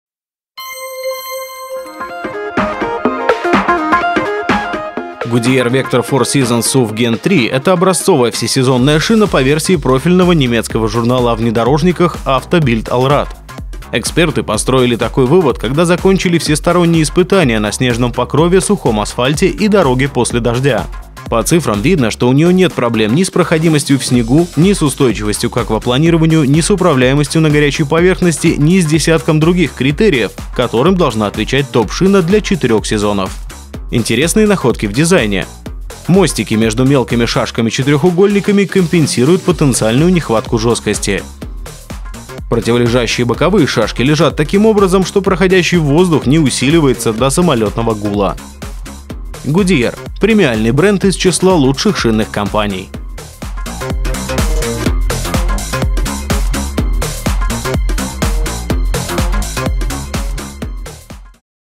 Goodyear Vector 4Seasons SUV Gen-3 ❄️☀️: обзор шины и отзывы ● Автосеть ●
Отзывы о всесезонной шине Goodyear Vector 4Seasons SUV Gen-3 и стоимость по размерам

Также по ссылке можно узнать детальные характеристики: рейтинги по евроэтикетке, страну производства, высоту протектора, вес и преимущественное покрытие.